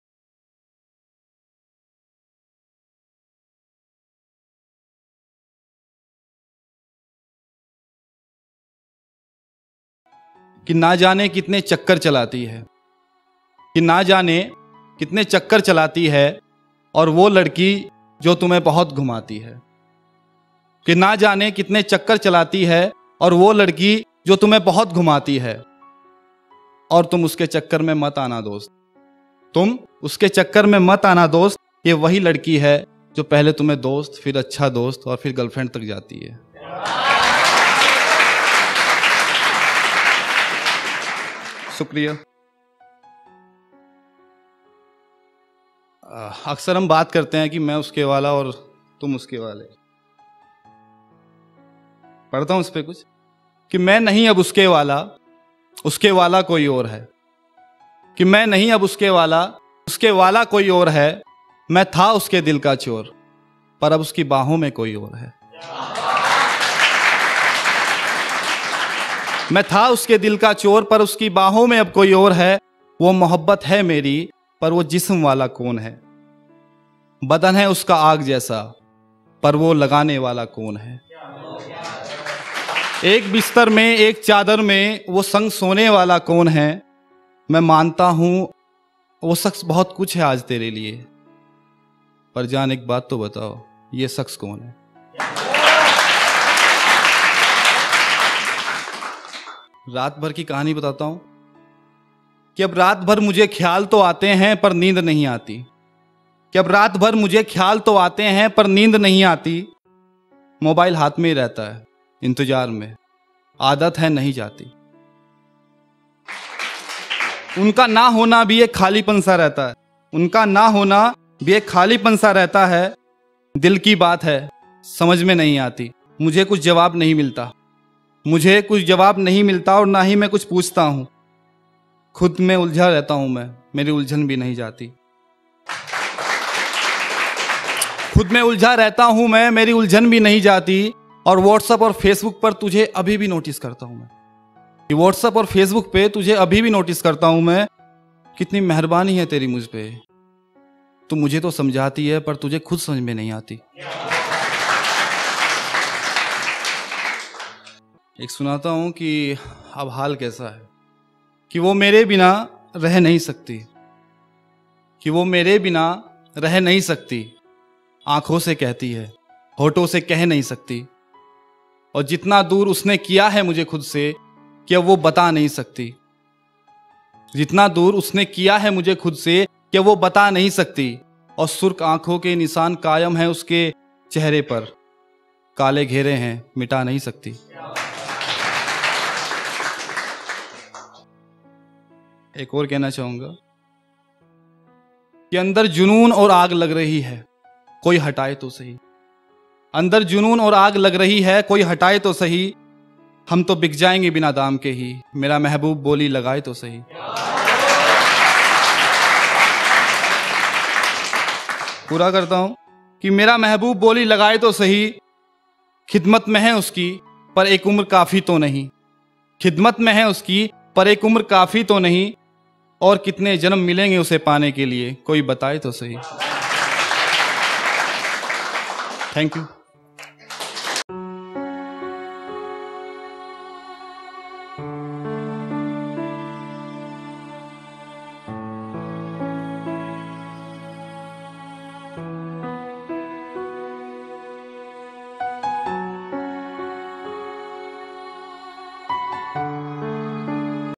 Naa Jane Kitne CHAKKAR CHALATI HAI | Poem by Harbeer Singh Arya | TRD Poetry | The Realistic Dice
TRD present poetry open mic in collaboration with ART BOX, metro Pillar Number 781, Land Mark Polo Garden, Dwarka Mod Metro Station, New Delhi. Thanks for having us ART BOX , venue was really amazing. If you are near by Dwarka and having interest interest in music and dance, do visit this place.